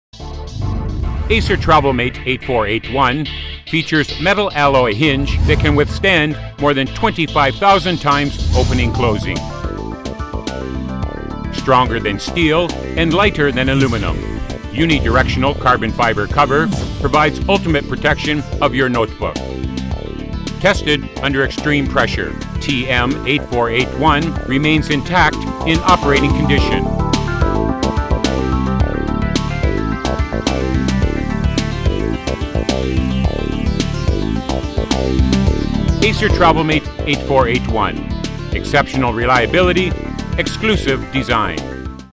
Acer TravelMate 8481 - Exceptional Reliability, Exclusive Design
Acer TravelMate 8481 features metal-alloy hinge that can withstand more than 25,000 times opening/closing. Stronger than steel and lighter than Aluminum, uni-directional carbon fiber cover provides ultimate protection of your notebook.
Tested under extreme pressure, TM8481 remains intact in operating condition.